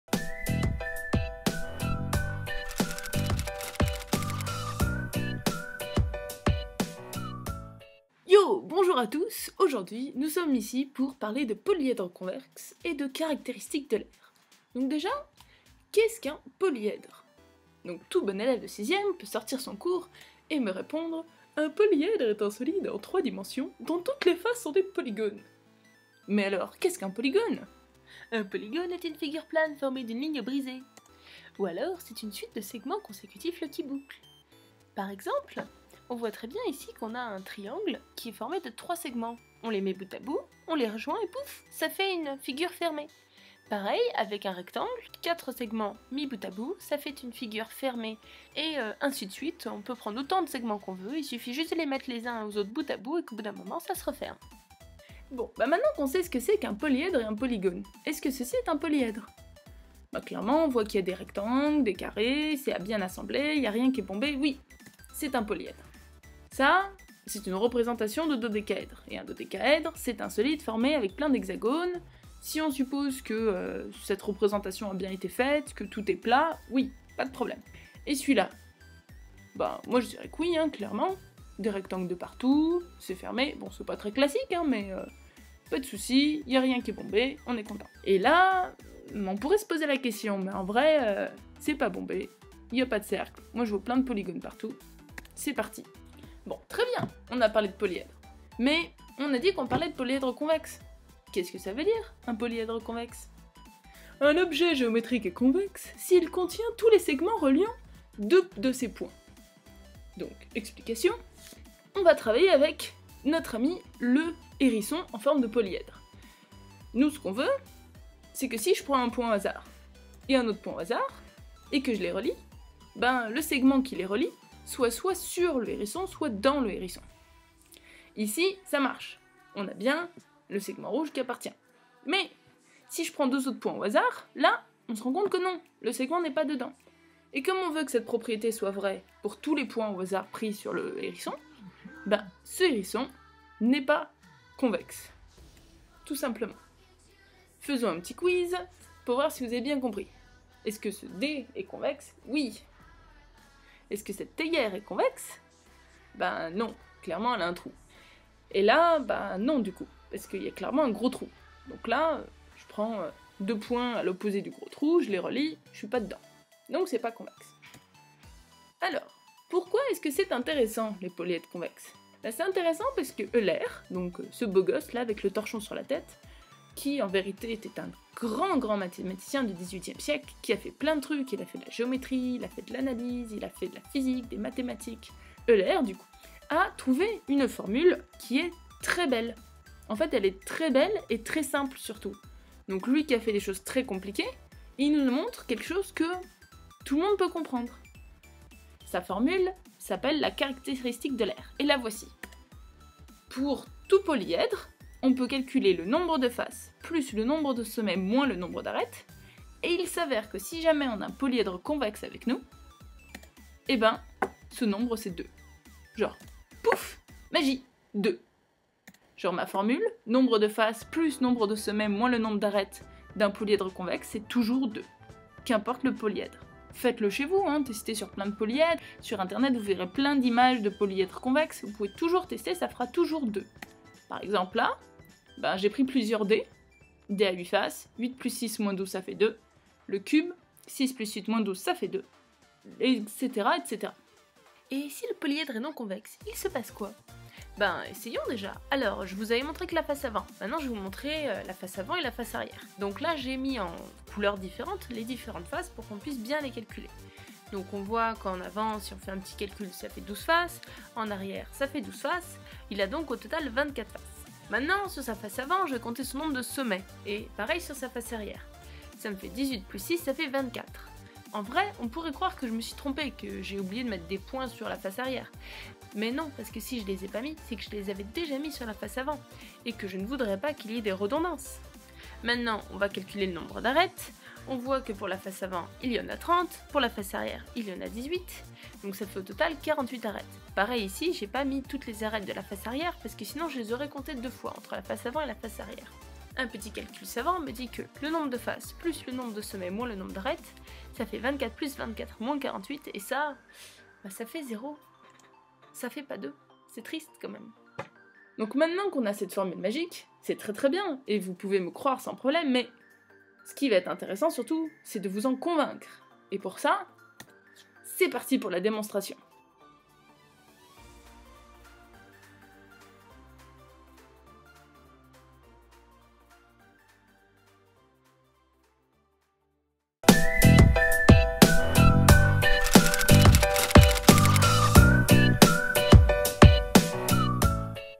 Polyèdre convexe et théorème d'Euler - Partie 1 - InterCycles
1ère partie présentant les polyèdres convexes et le théorème d'Euler. "Un polyèdre est convexe si toutes ses diagonales sont entièrement contenues dans son intérieur".